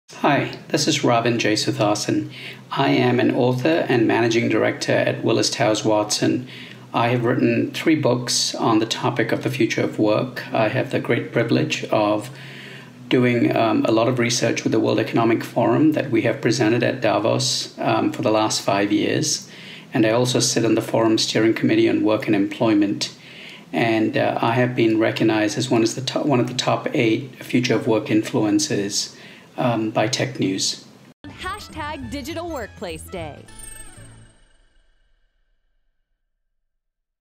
Redefining productivity for a digital workplace
Ravin Jesuthasan, Managing Director, Willis Towers Watson. Panelist for Session 7: Redefining productivity for a digital workplace.

This session was a part of Digital Workplace Day, a 24-hour tweetathon on Oct 6th, presented by The Digital Workplace.